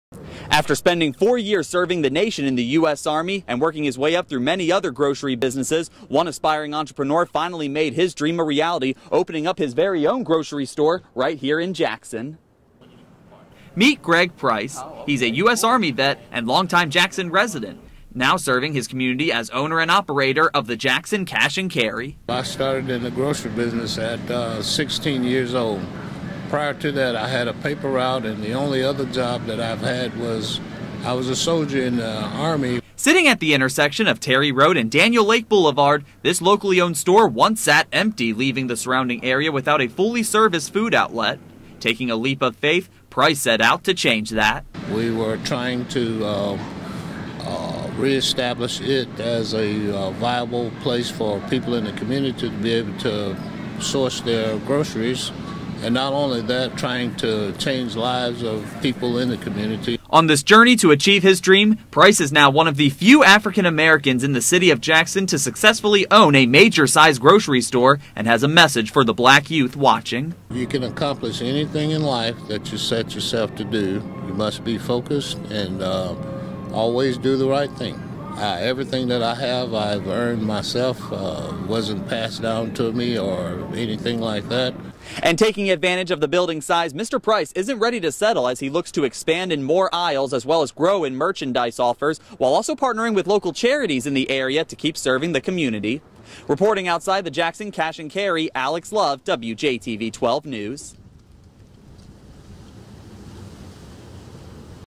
From Soldier to local Business Owner, the journey of serving others
Greg Price is an US Army vet and longtime Jackson resident, now serving his community as owner and operator of the Jackson Cash and Carry.